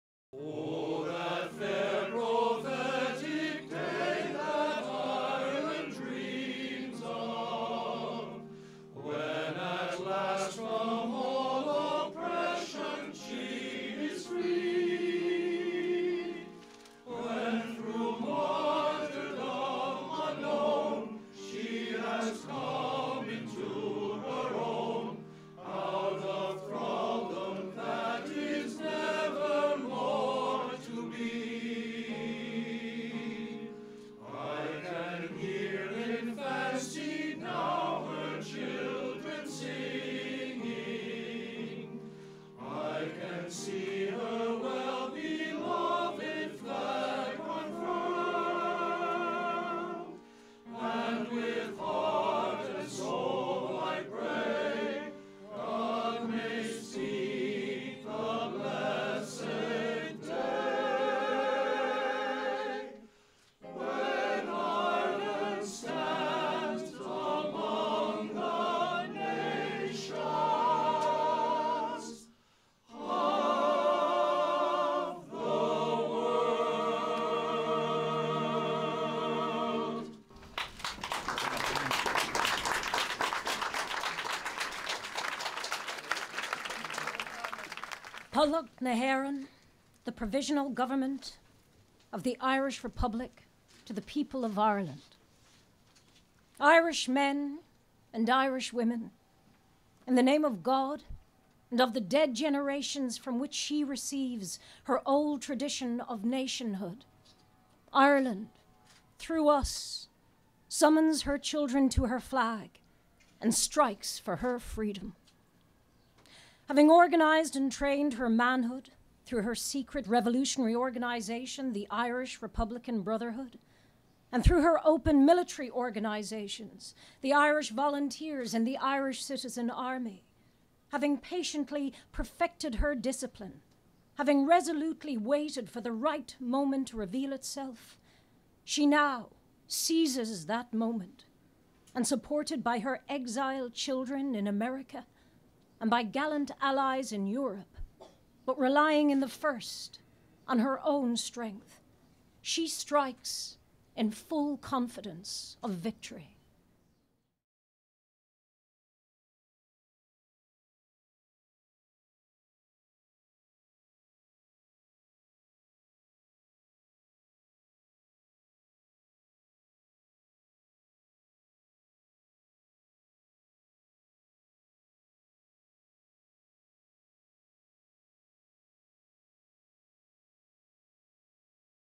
Reading of the Proclamtion by Lisa Dwan
Glucksman Ireland House NYU commemorates the centennial of the Proclamation of the Irish Republic.  Reading of the Proclamation by Lisa Dwan, musical performance by Friendly Sons of St. Patrick Glee Club.